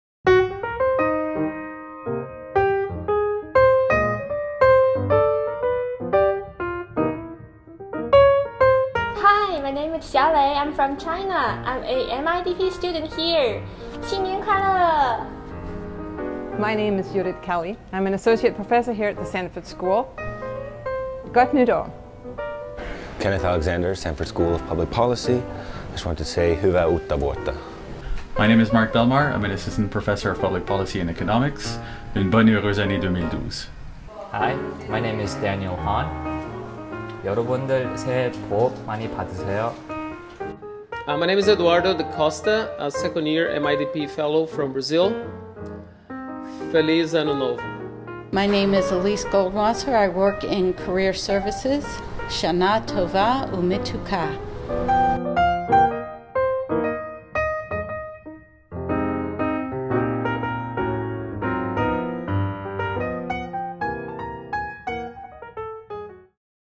How to Say "Happy New Year" in 7 Languages (from the Duke Sanford School of Public Policy)
"Happy New Year" in Finnish, Danish, Korean, Quebecois (French), Mandarin, Brazilian Portuguese, and Hebrew, from the staff, students and faculty of the Duke Sanford School of Public Policy.